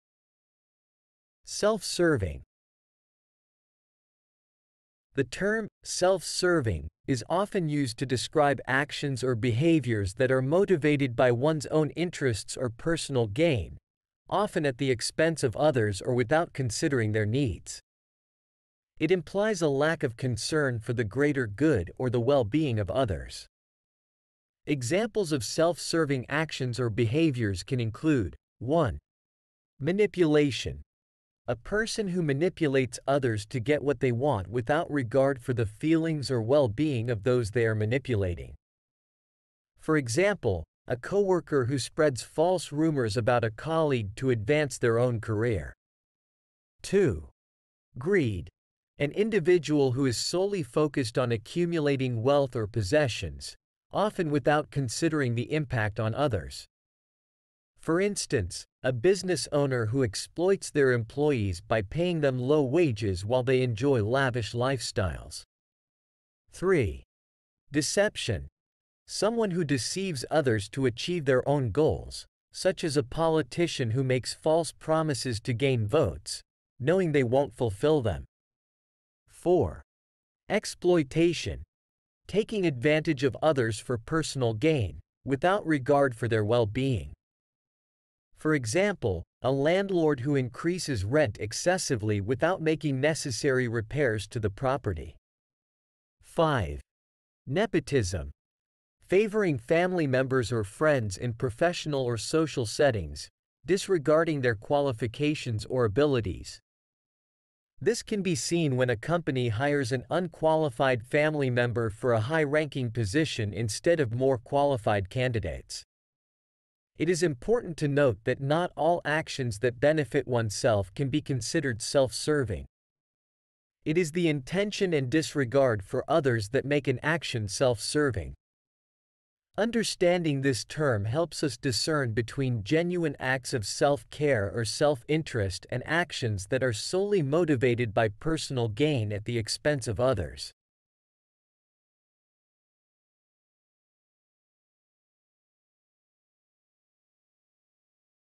Self-serving
The term "self-serving" is often used to describe actions or behaviors that are motivated by one's own interests or personal gain, often at the expense of others or without considering their needs. It implies a lack of concern for the greater good or the well-being of others.
Examples of self-serving actions or behaviors can include:
1. Manipulation: A person who manipulates others to get what they want without regard for the feelings or well-being of those they are manipulating. For example, a coworker who spreads false rumors about a colleague to advance their own career.
2. Greed: An individual who is solely focused on accumulating wealth or possessions, often without considering the impact on others. For instance, a business owner who exploits their employees by paying them low wages while they enjoy lavish lifestyles.
3. Deception: Someone who deceives others to achieve their own goals, such as a politician who makes false promises to gain votes, knowing they won't fulfill them.
4. Exploitation: Taking advantage of others for personal gain, without regard for their well-being. For example, a landlord who increases rent excessively without making necessary repairs to the property.
5. Nepotism: Favoring family members or friends in professional or social settings, disregarding their qualifications or abilities. This can be seen when a company hires an unqualified family member for a high-ranking position instead of more qualified candidates.
It is important to note that not all actions that benefit oneself can be considered self-serving. It is the intention and disregard for others that make an action self-serving. Understanding this term helps us discern between genuine acts of self-care or self-interest and actions that are solely motivated by personal gain at the expense of others.